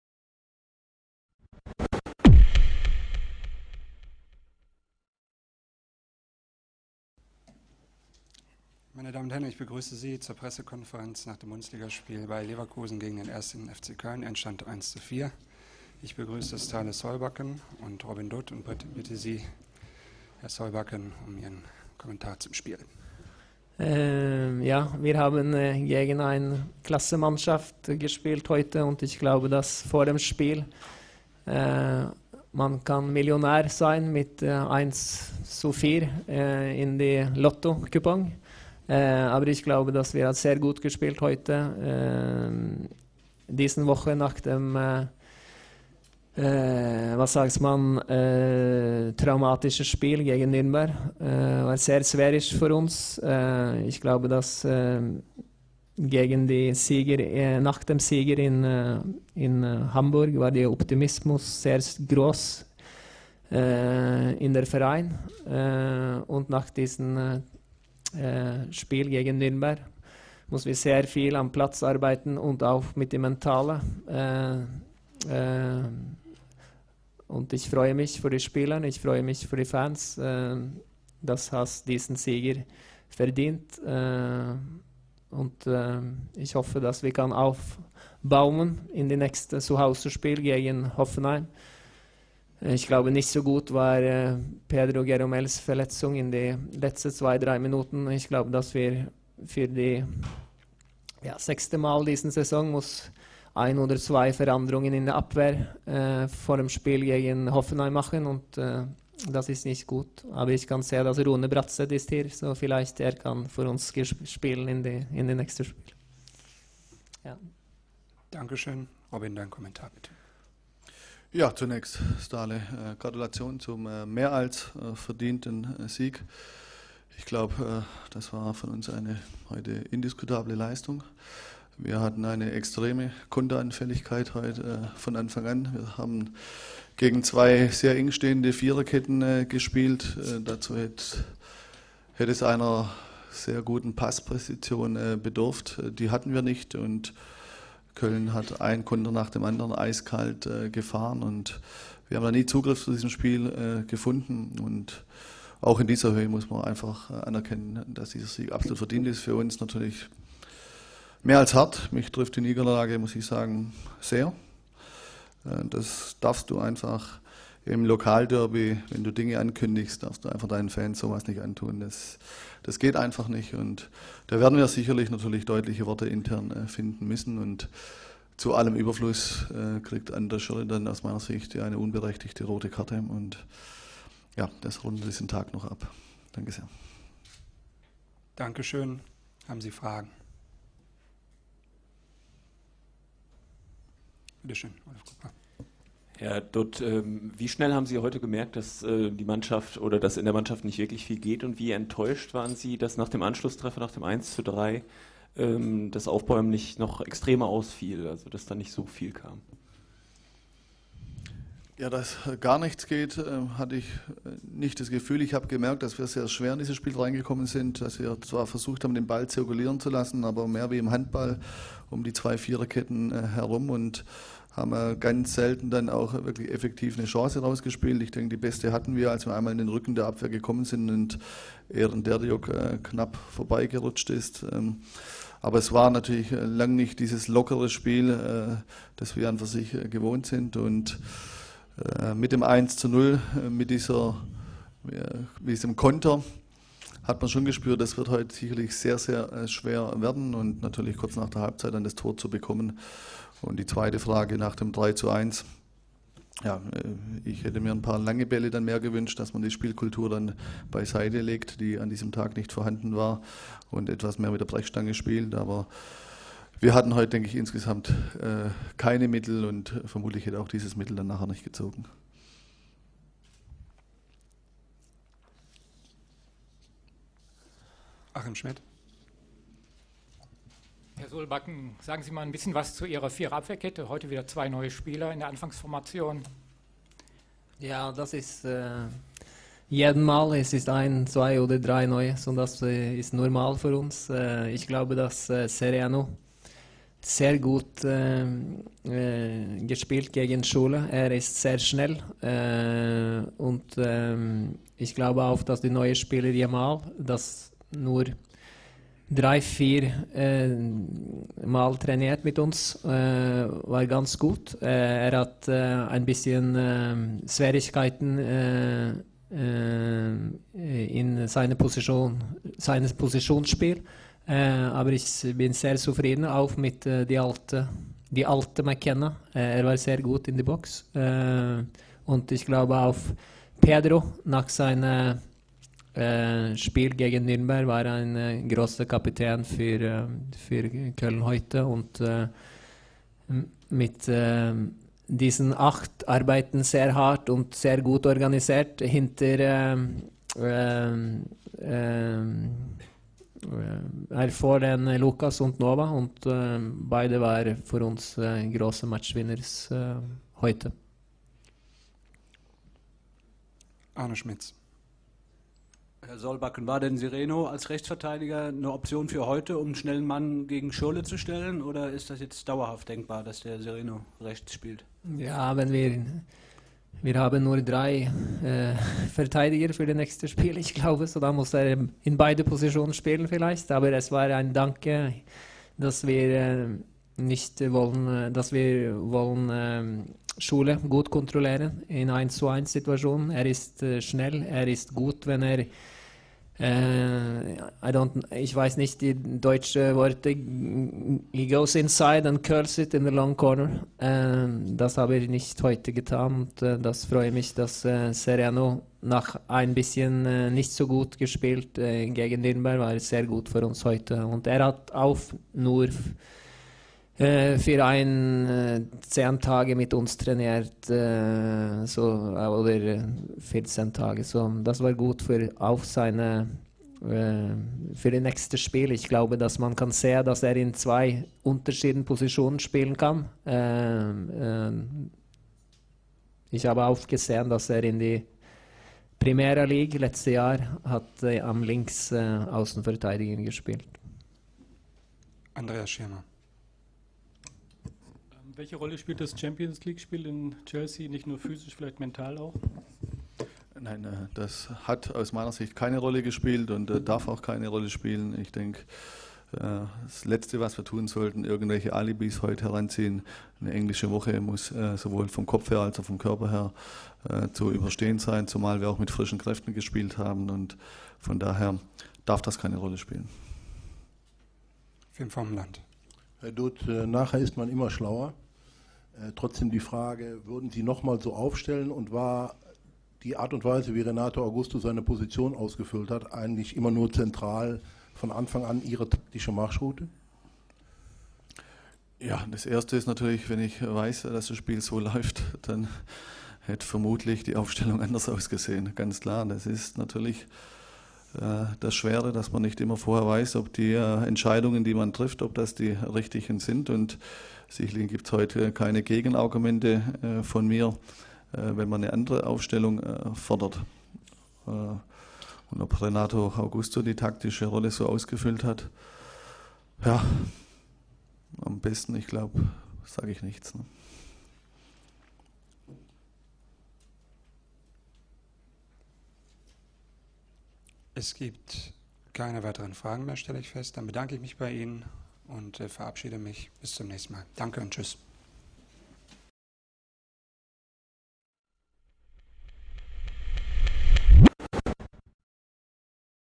PK nach dem Derbysieg
PK nach dem Derbysieg Bayer04Leverkusen vs. 1.FC Köln.Endstand 1:4.
Der zweite Saisonsieg für unsere Jungs und die haben richtig gut gespielt
und verdient gewonnen!!!